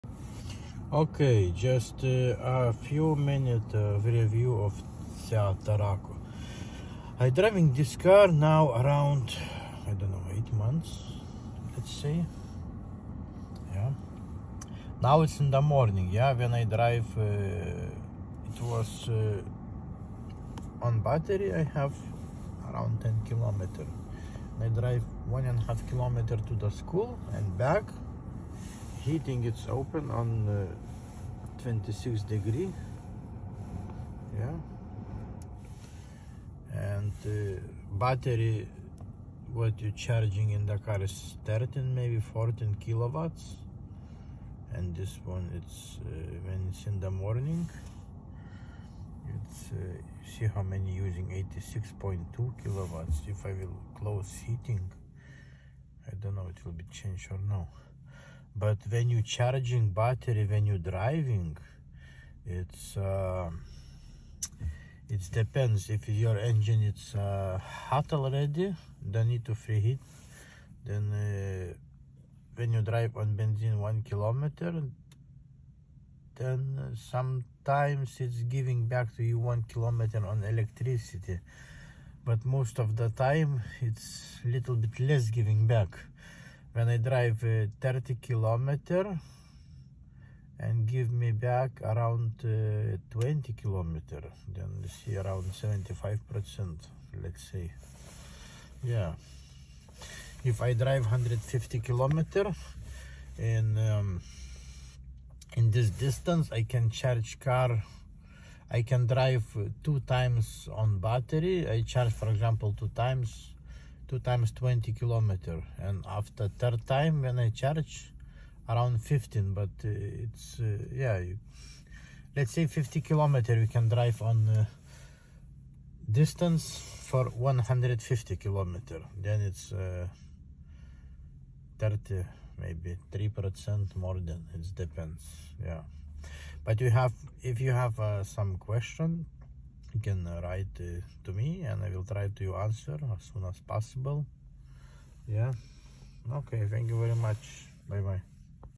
Seat Tarraco e-hibrid 1,5 phev plug in short Rewievdriving almost 1 year
Battery consumption Kw / 100km
charging time will travel the distance at different times and other issues
ask for comments
I will try to answer as soon as possible
or write by email